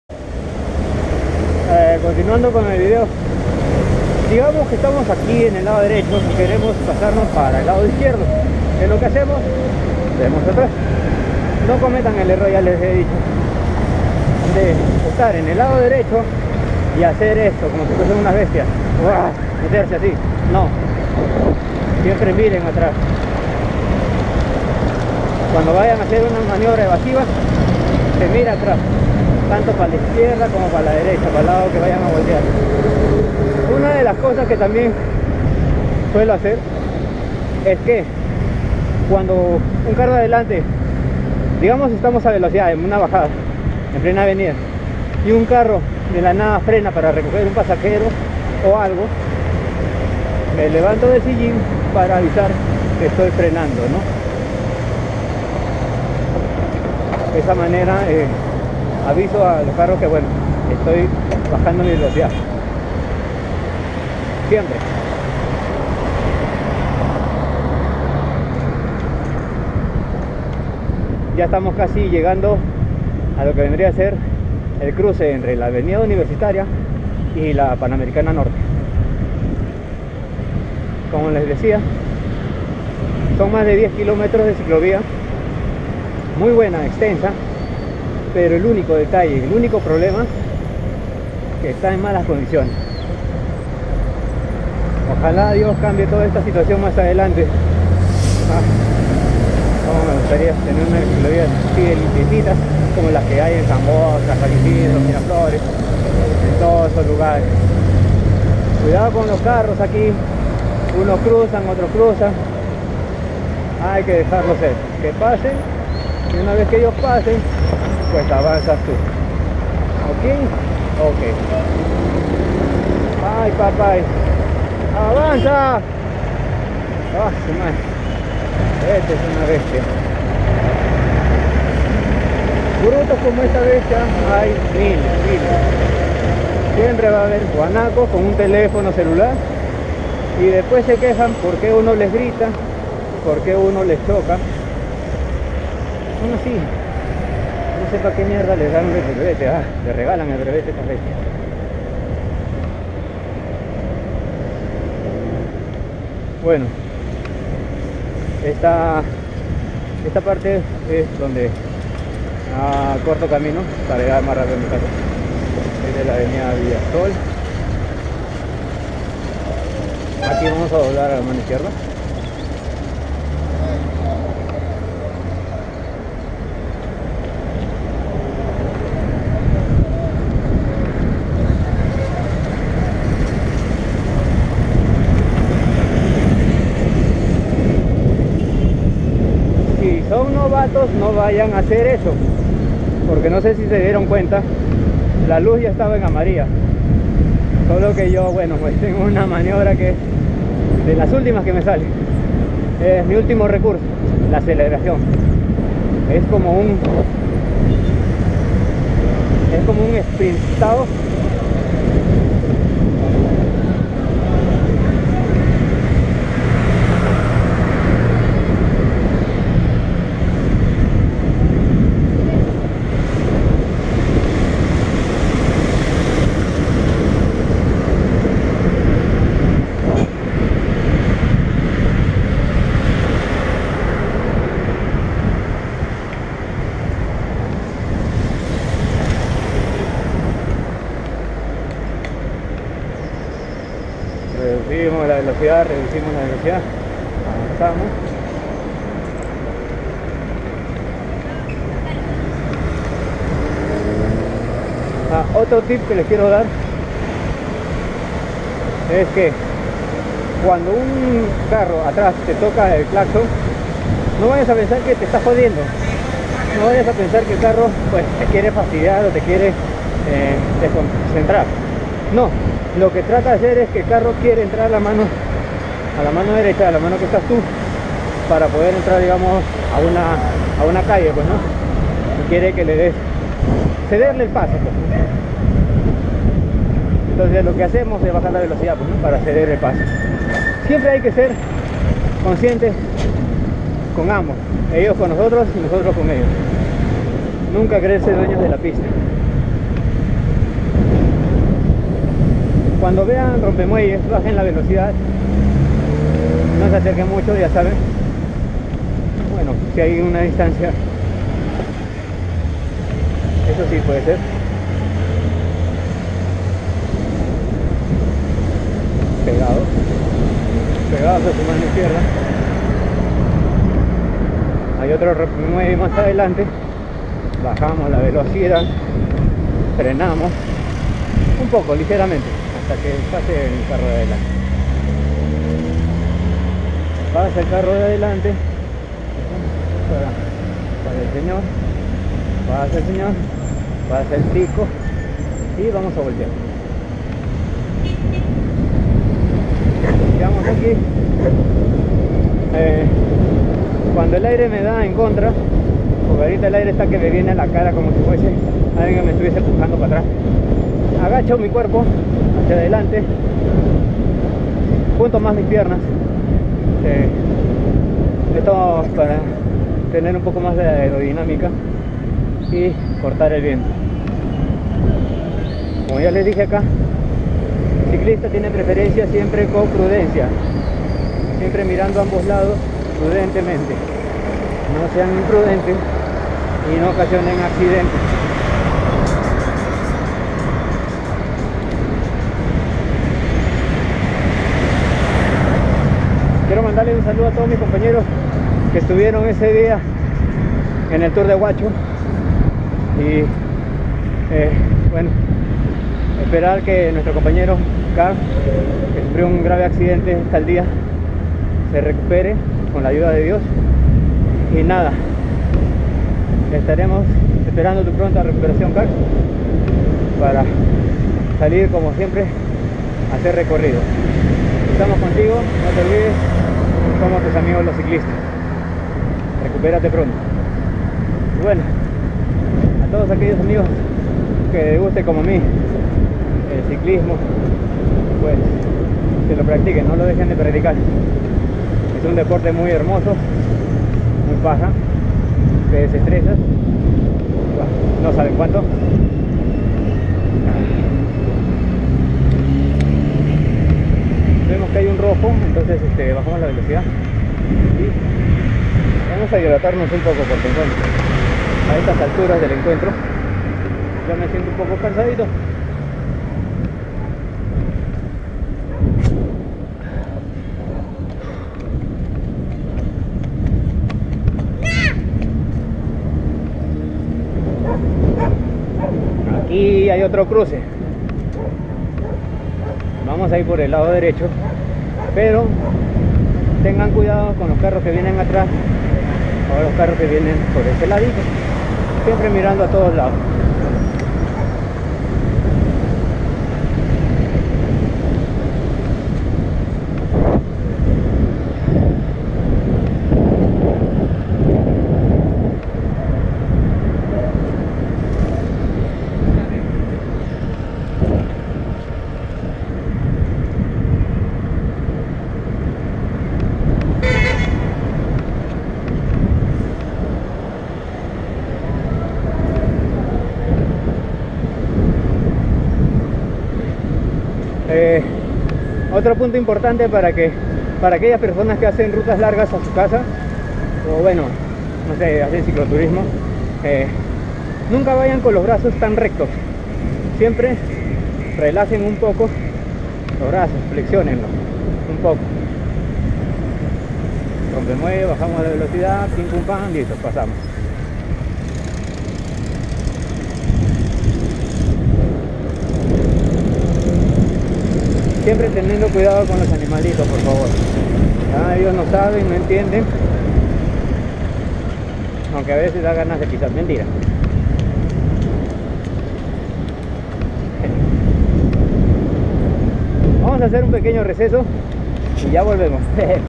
ciclismo urbano - consejos utiles (parte 4)
si quieres trasladarte de tu casa a tu trabajo o ir de un punto a otro x la ciudad y aun eres principiante te dejo estos consejos para que tengas en cuenta y puedas ir con cuidado a tu destino ...recuerda si t gusto el video y t fue util dale like :D gracias y suscribete para seguir compartiendo mas videos